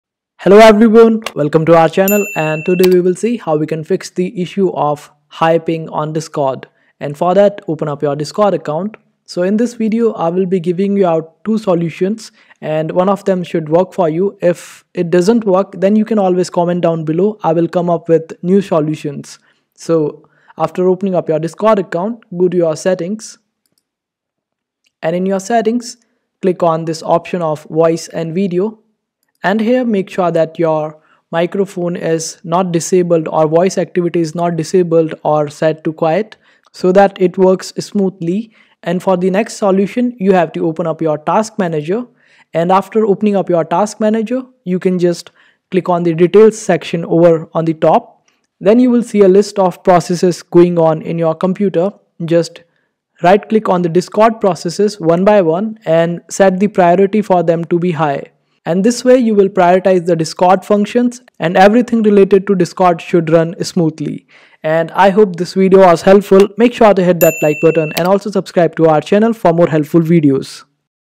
How To Fix High Discord Ping (SIMPLE!)
How To Fix High Discord Ping. It’s a very easy tutorial, I will explain everything to you step by step.